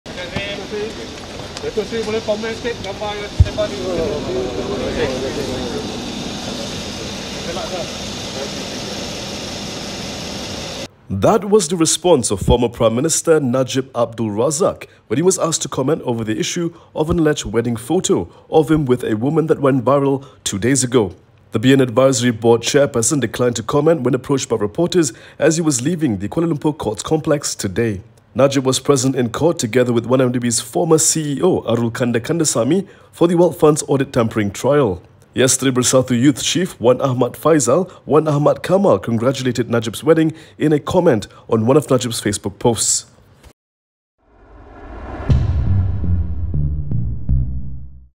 Najib declines to comment on viral 'marriage' photo
Former prime minister Najib Abdul Razak declined to comment over the issue of an alleged "wedding" photo of him with a woman that went viral two days ago.

The BN advisory board chairperson made this response when approached by the media as he was leaving the Kuala Lumpur Courts Complex today.

Najib, who was a finance minister, was present in court together with 1MDB’s ex-CEO, Arul Kanda Kandasamy, for the 1MDB audit tampering trial.

He did not answer queries by the media about the photo as he was about to enter a car to leave the complex.